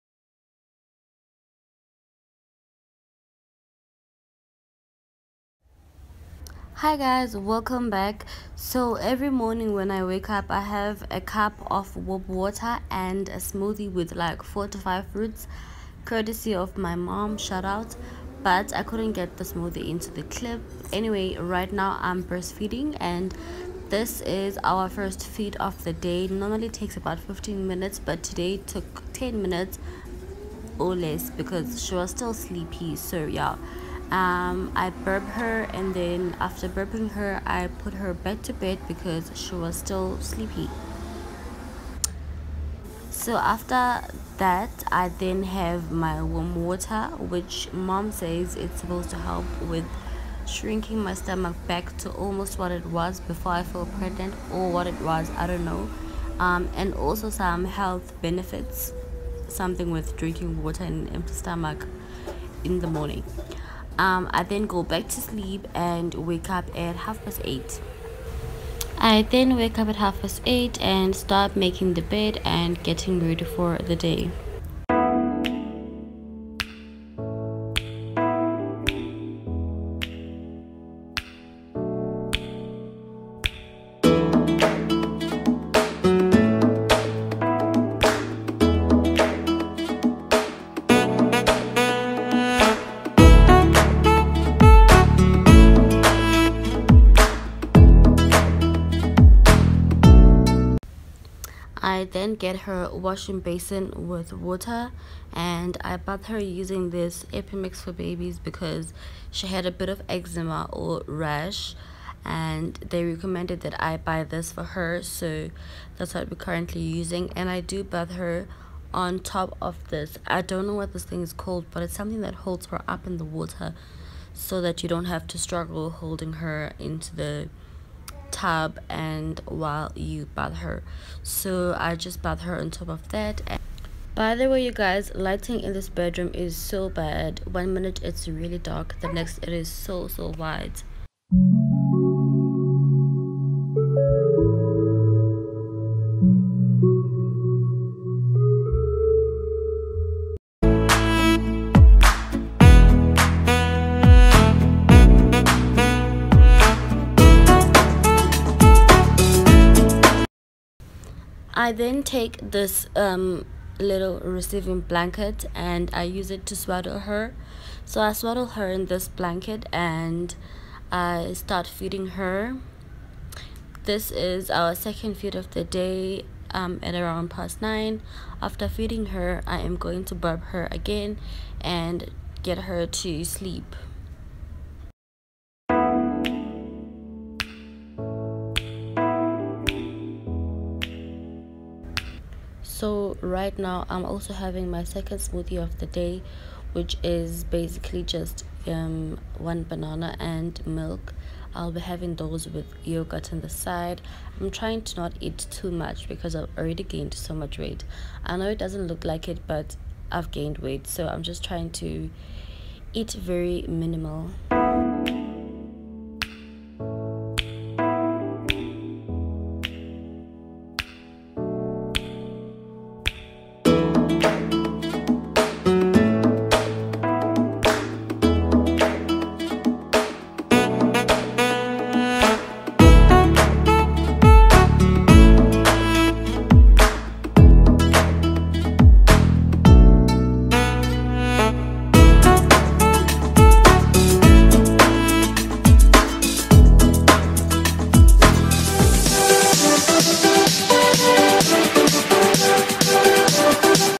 Morning routine with a 7 weeks old baby || Roundy Mvumvu || South African YouTuber
Hey guys. I’m back again today with our morning routine with my baby.